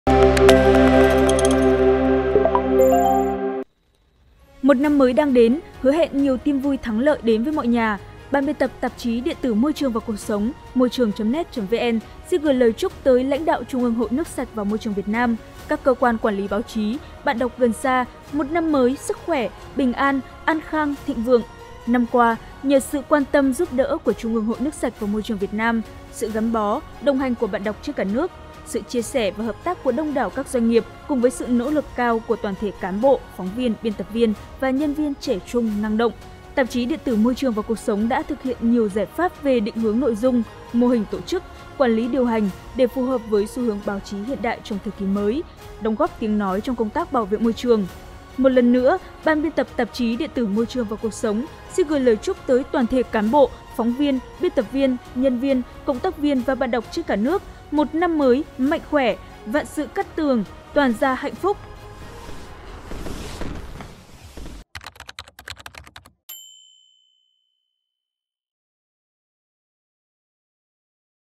Tạp chí Điện tử Môi trường và Cuộc sống - Moitruong.net.vn - Chúc mừng năm mới 2024
Một năm mới đang đến, hứa hẹn nhiều tin vui thắng lợi đến với mọi nhà! Ban Biên tập Tạp chí Điện tử Môi trường và Cuộc sống - Moitruong.net.vn xin gửi lời chúc tới lãnh đạo Trung ương Hội Nước sạch và Môi trường Việt Nam, các cơ quan quản lý báo chí, bạn đọc gần xa một năm mới Sức khỏe - bình an, an khang - thịnh vượng!